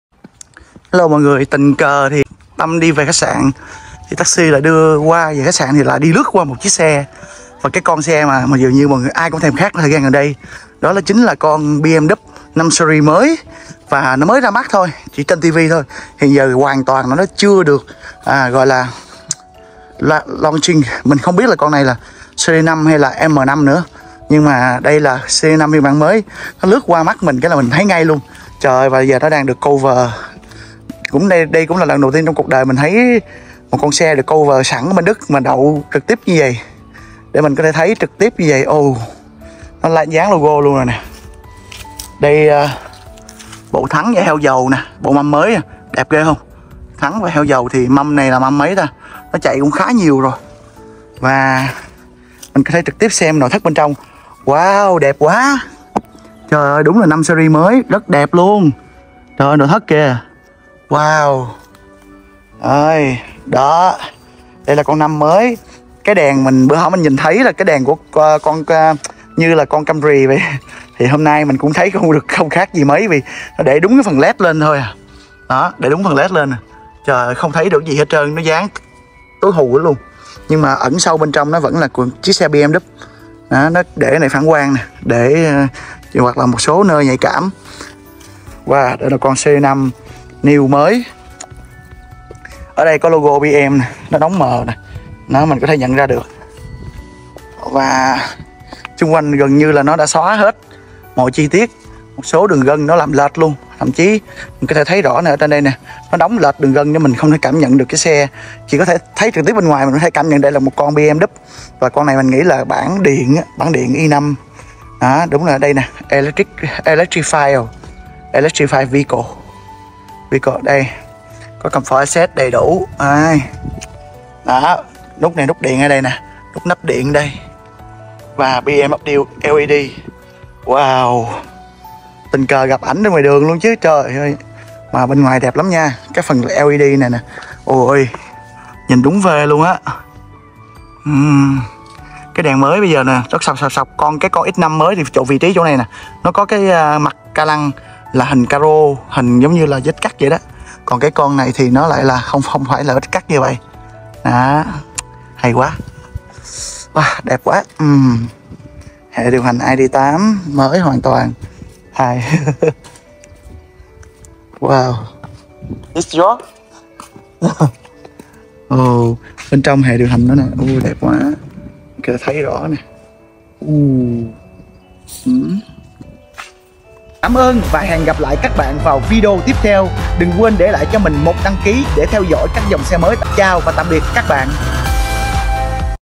BMW i5 all new 2023 tại Đức | BMW 5 series mới đại diện sự hoàn hảo của business man| Tâm béo bmw
Chiếc xe được mình tình cờ bắt gặp tại Đức khi đang chạy thử , xe vẫn chưa được chính thức bán nên mọi điều của chiếc xe chỉ được thấy qua video quảng cáo và những chi tiết nổi bật vẫn chưa được giới thiệu